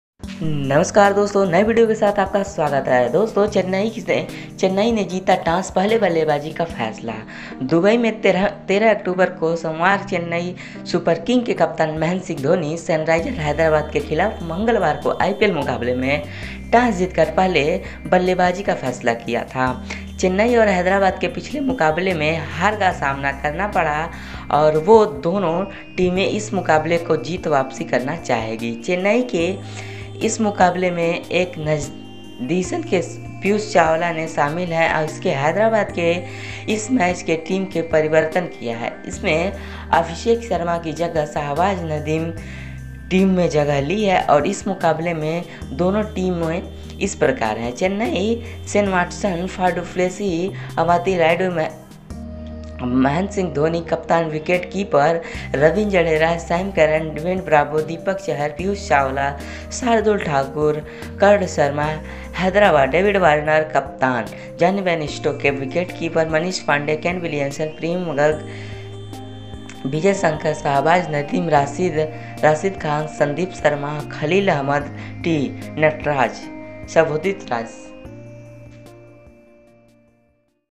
चेन्नई ने जीता टॉस पहले बल्लेबाजी करने का फैसला
दुबई, 13 अक्टूबर (सन्मार्ग) चेन्नई सुपर किंग्स के कप्तान महेंद्र सिंह धोनी ने सनराइजर्स हैदराबाद के खिलाफ मंगलवार को आईपीएल मुकाबले में टॉस जीतकर पहले बल्लेबाजी का फैसला किया।
चेन्नई और हैदराबाद को पिछले मुकाबलों में हार का सामना करना पड़ा था और दोनों टीमें इस मुकाबले को जीत वापसी करना चाहेंगी। चेन्नई ने इस मुकाबले के लिए एन जगदीशन की जगह पीयूष चावला को शामिल किया है। हैदराबाद ने भी इस मैच के लिए टीम में एक परिवर्तन किया है। उसने अभिषेक शर्मा की जगह शाहबाज नदीम को टीम में जगह दी है।
इस मुकाबले के लिए दोनों टीमें इस प्रकार हैः
चेन्नईः शेन वाटसन, फॉफ डू प्लेसिस, अंबाटी रायुडू, महेंद्र सिंह धोनी (कप्तान और विकेटकीपर), रवींद्र जडेजा, सैम करेन, ड्वेन ब्रावो, दीपक चाहर, पीयूष चावला, शार्दुल ठाकुर, कर्ण शर्मा
हैदराबादः डेविड वार्नर (कप्तान), जॉनी बेयरस्टो (विकेटकीपर), मनीष पांडे, केन विलियम्सन, प्रियम गर्ग, विजय शंकर, शाहबाज नदीम, राशिद खान, संदीप शर्मा, खलील अहमद और टी नटराजन।